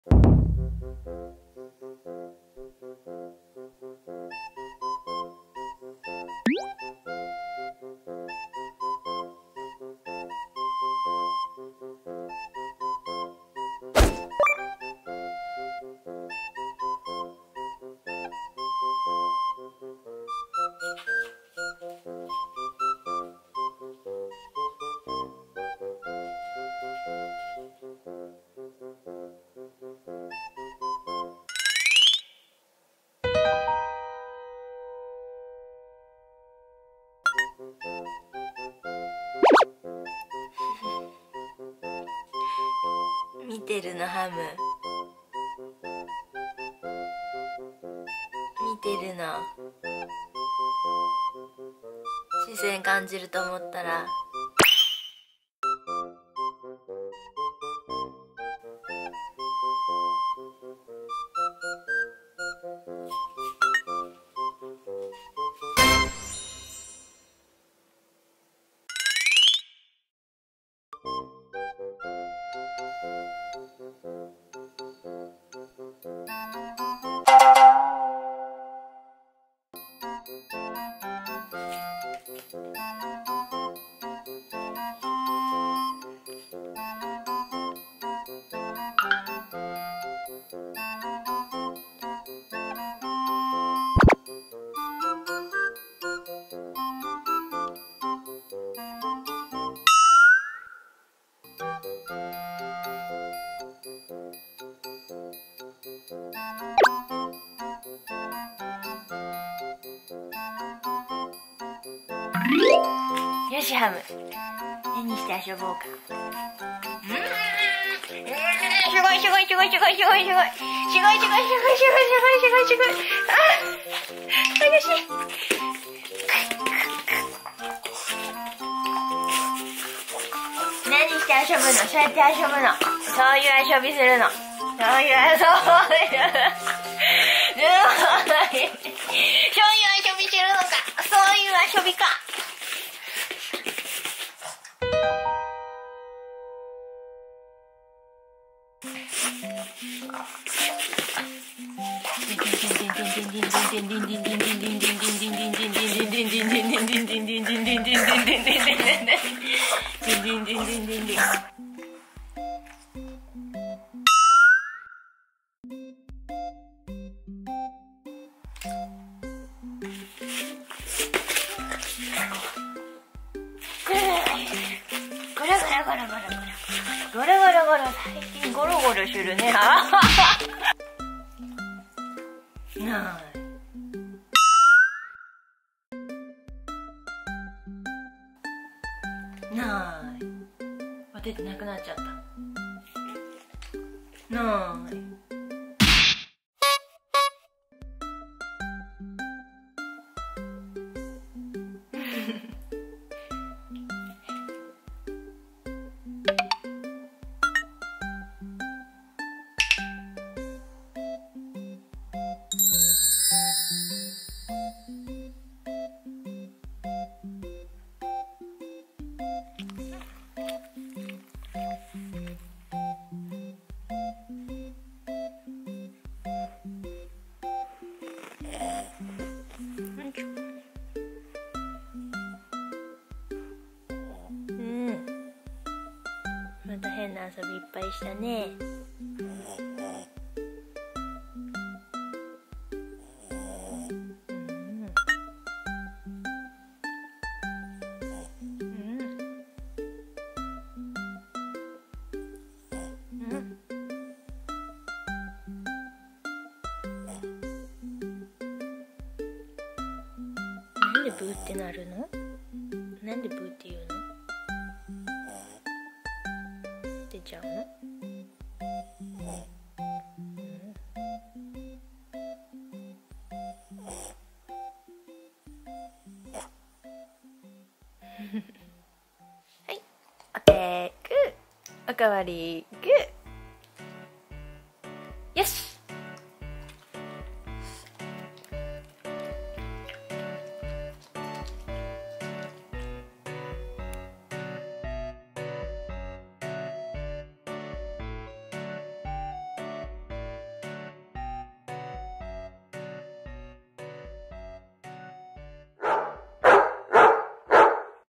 半径2メートル以上ママから離れたくないボストンテリア
使用カメラ Used camera
SONY RX100M6

フレンチブルドッグ じゃないよ|ω･｀) Not a French bulldog

ボストンテリアのハムのゆるーい動画です(๑ˇεˇ๑)•*¨*•.¸¸♪